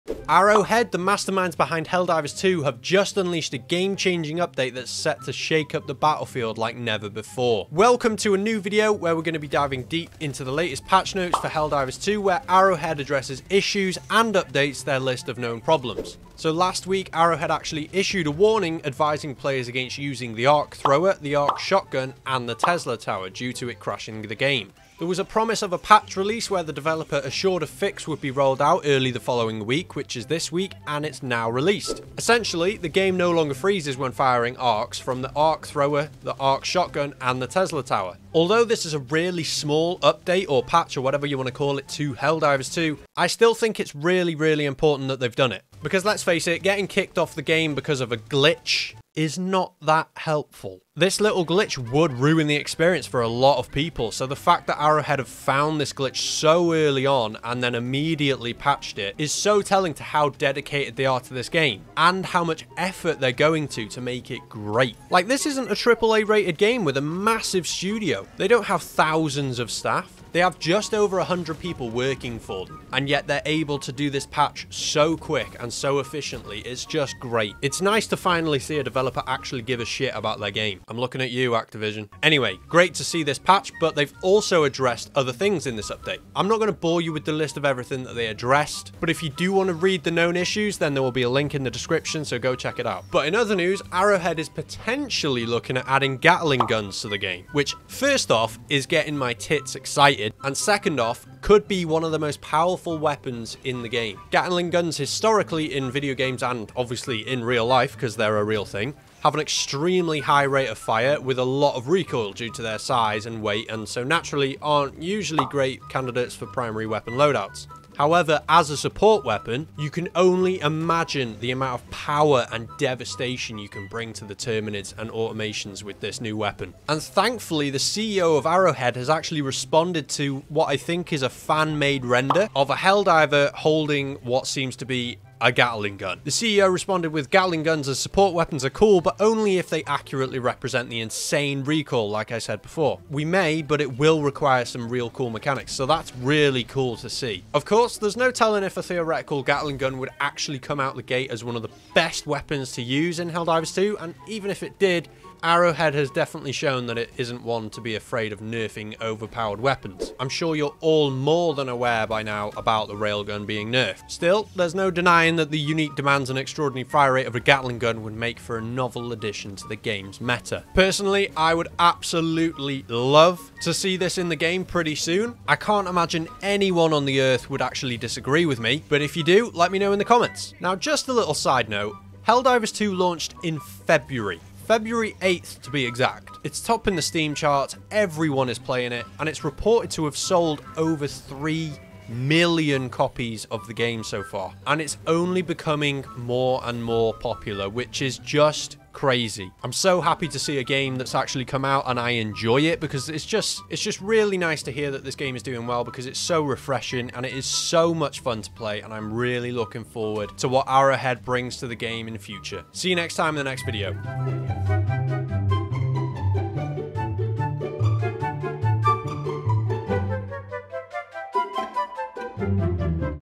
Helldivers 2 Patch is a GAME CHANGER!
We think the recent helldivers 2 patch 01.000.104 is a GAME CHANGER! Last week, Arrowhead told players not to use the Arc Thrower, Arc Shotgun, and Tesla Tower because they caused game crashes. At the time, the developer promised a patch addressing this would be released early this week, and now it’s out -  Arrowhead also issued an updated list of known issues it’s working to address!

Support The Milk Bar Podcast (we are going to be homeless soon if no-one helps):

Follow all of these or we will hunt you down